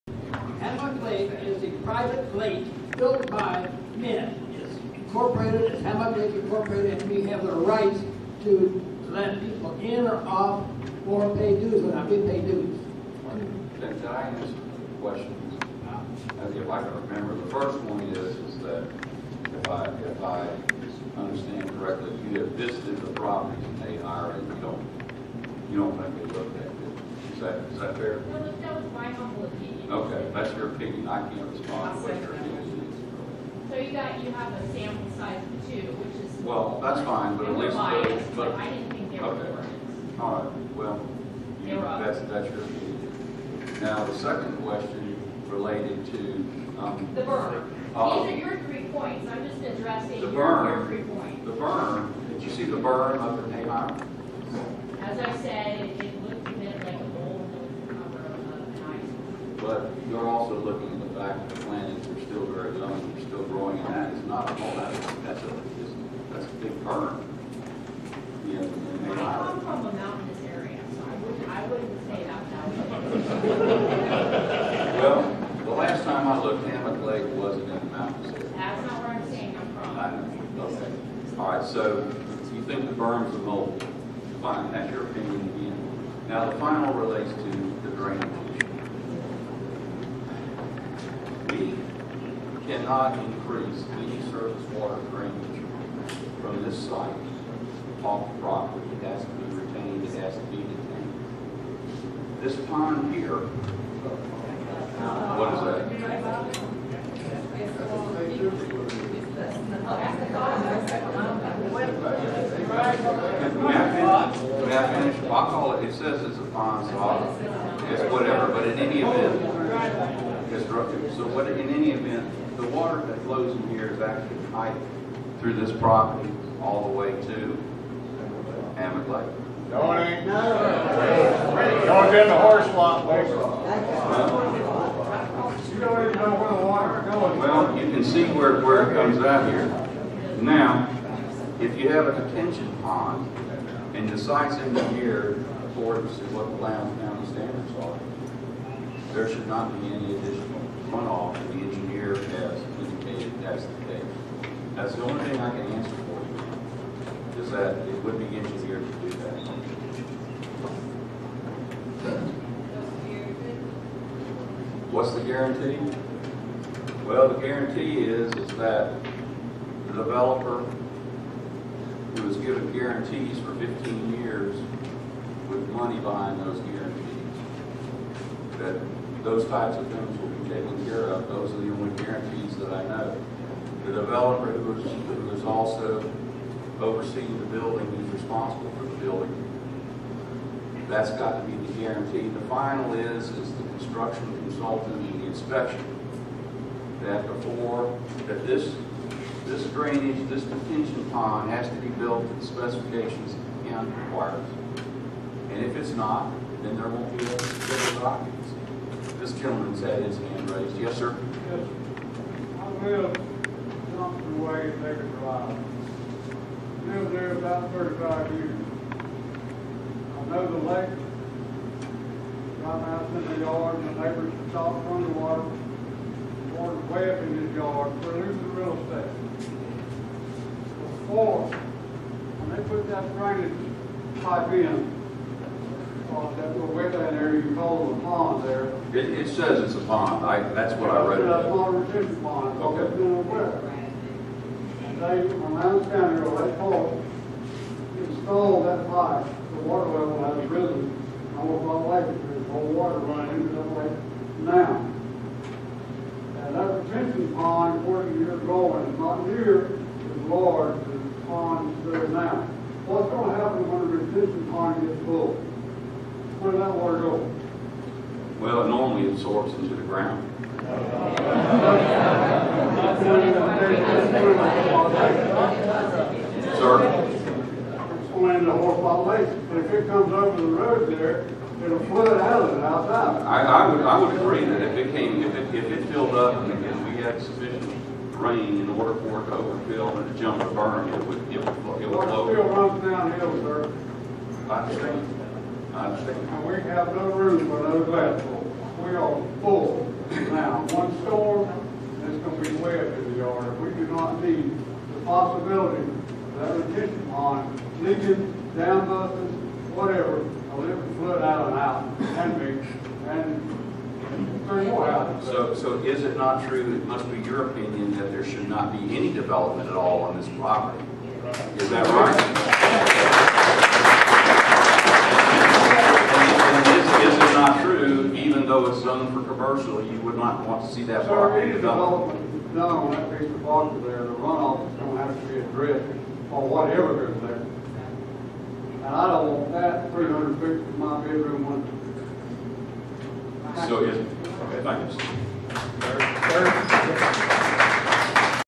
Hammock Lake is a private lake --Lake Park Brookhaven Rezoning Public Hearing 2014-04-28
Rezoning Public Hearing, Lake Park City Council (Lake Park)
 Lake Park, Lowndes County, Georgia, 28 April 2014.